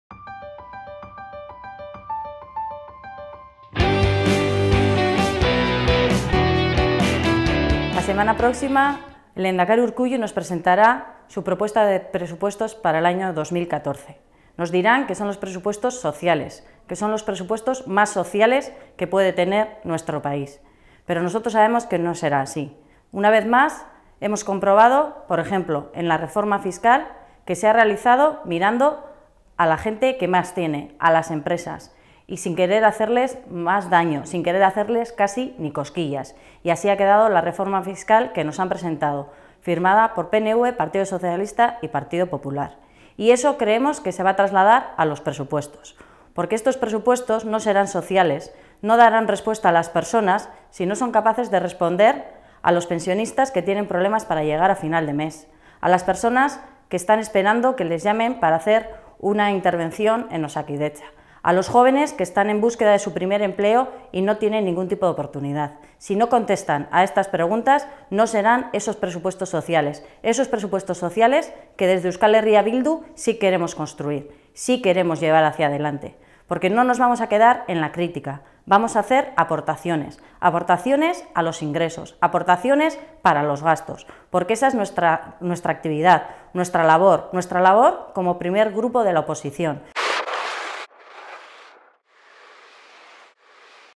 Prioridad para los presupuestos de 2014: que cubran las necesidades laborales y sociales de todos
EH Bildu ha ofrecido esta mañana una rueda de prensa en Bilbao, fijando las prioridades de la coalición para los presupuestos del 2014 del Gobierno Vasco. El ejecutivo de Urkullu presentará el próximo martes las cuentas que determinarán la inversión pública durante el 2014. La coalición le ha advertido de que "no debe dar la espalda a las personas que lo están pasando mal, a las miles de familias que viven sin recursos, a las personas que se han quedado sin ayudas sociales, a los trabajadores y trabajadoras que han visto perder sus derechos laborales, a las personas que viven en la exclusión social"